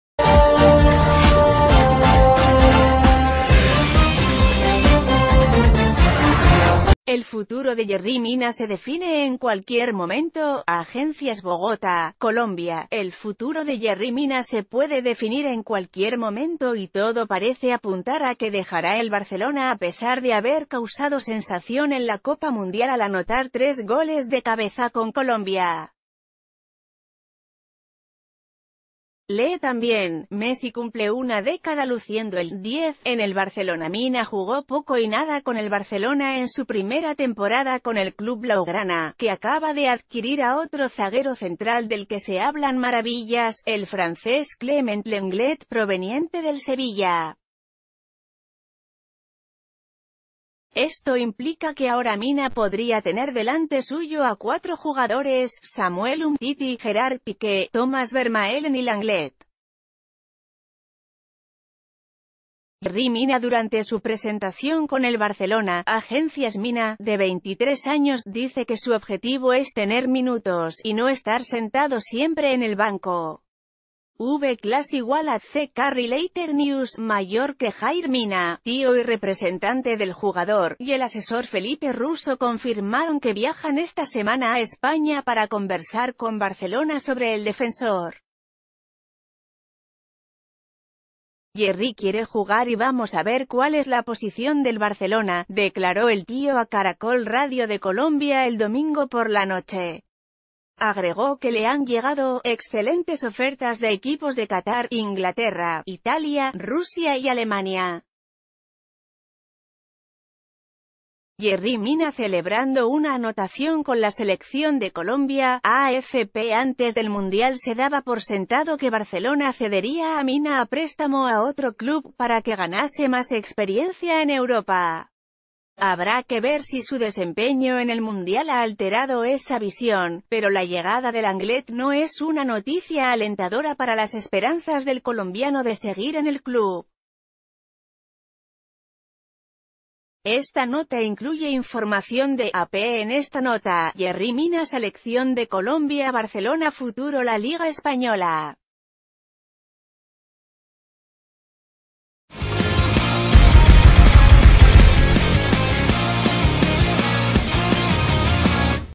El futuro de Yerry Mina se define en cualquier momento
El futuro de Yerry Mina se define en cualquier momento/Agencias Bogota, Colombia.- El futuro de Yerry Mina se puede definir en cualquier momento y todo parece apuntar a que dejará el Barcelona a pesar de haber causado sensación en la Copa Mundial al anotar tres goles de cabeza con Colombia. Lee tamb...